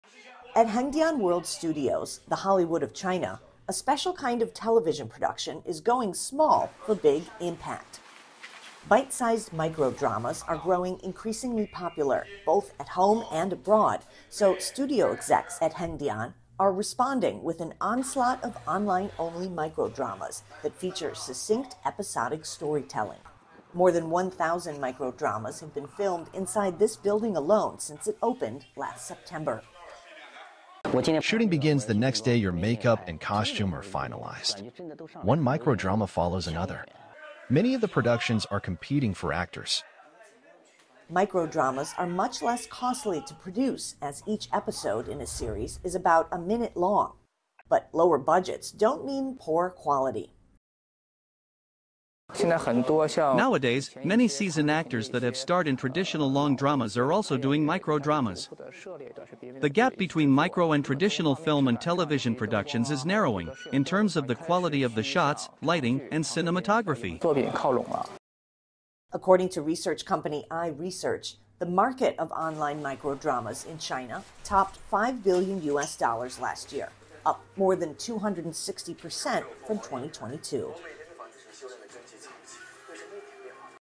Short and fast: Micro drama industry growing strong in China
Bite-sized micro dramas are growing increasingly popular in China, both at home and abroad, so studio execs at Hengdian are responding with an onslaught of online-only micro drams that feature succinct episodic storytelling.

More than 1,000 micro dramas have been filmed inside this building alone since it opened last September.

According to research company iResearch, the market of online micro dramas in China topped five billion U.S. dollars last year, up more than 260 percent from 2022.